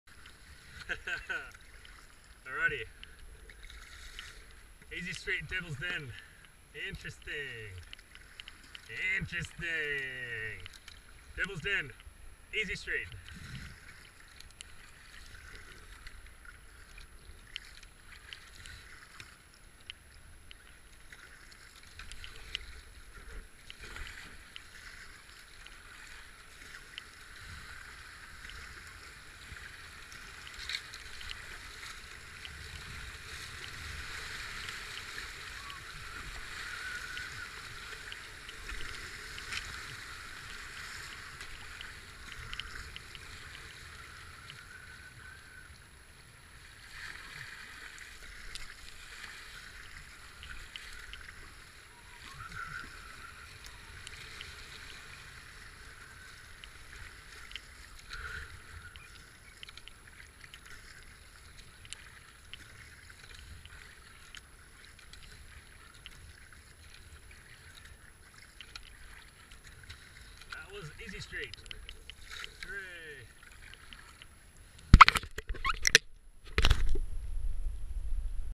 Avon Descent 2023 - 22 Day2 Easy Street
The predicted water level in the TiTrees was 0.6m and the Walyunga gauge is currently not operational doh!
The water levels were rising thru the day and by the evening they were 0.7m, rain from Friday still coming in I guess!